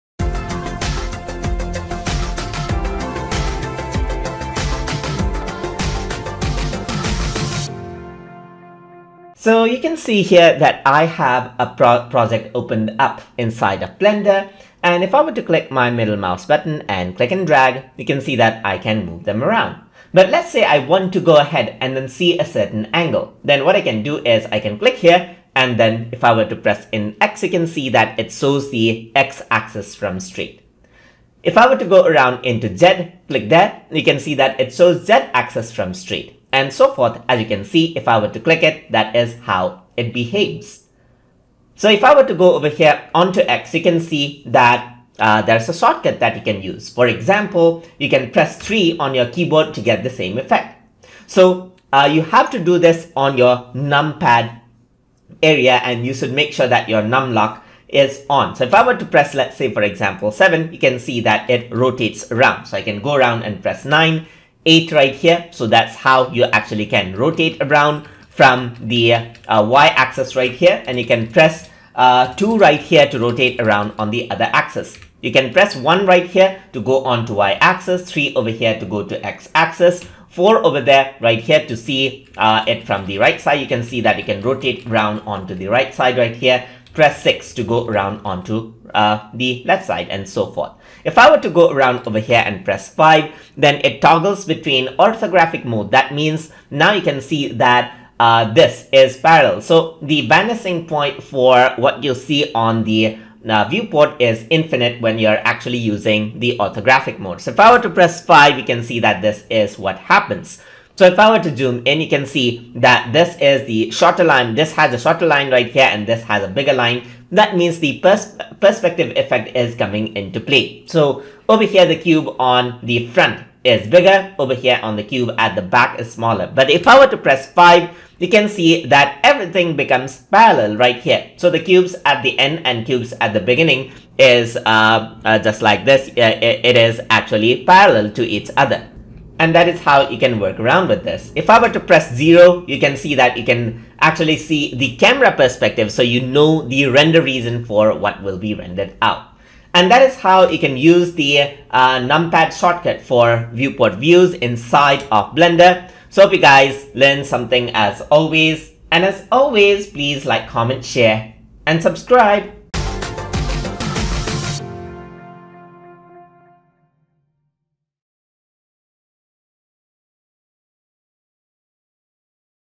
Blender Tutorial - Lesson 22 - Numpad Shortcut for Viewport Views
In this tutorial, we will be discussing about Numpad Shortcut for Viewport Views in Blender.